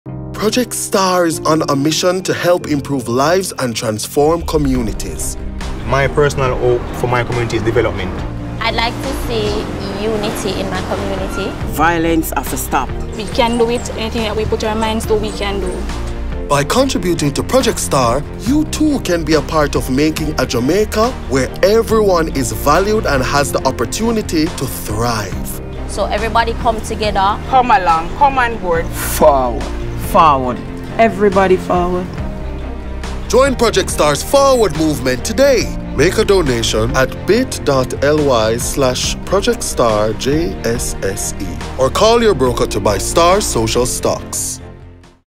Everybody FAHWUD - Donate to Project STAR
Project STAR believes that with collective action, we can transform Jamaica into a place where everyone is valued and has the opportunity to thrive.

You can help Project STAR do even more:
- OR Contact your broker to purchase STAR's Social Stock.
Share price is J$1 with a minimum purchase of 500 units.